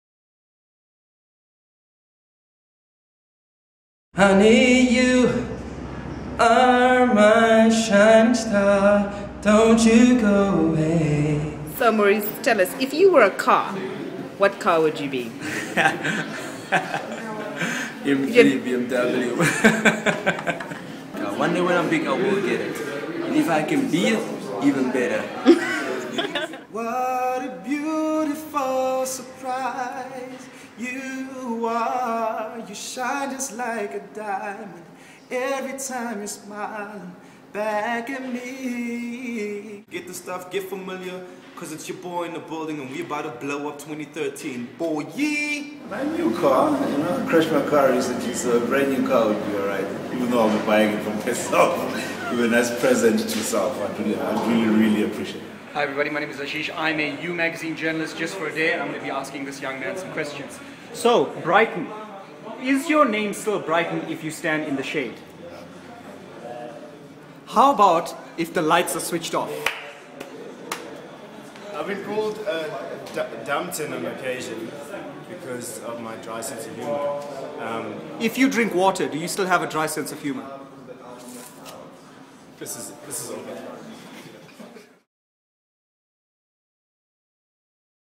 What you may or may not know about our hot hunks
What would be the greatest Christmas gift ever? Or what kind of car would you be? Some hidden and not so hidden talents galore! Meet the sexy guys you know from the small screen.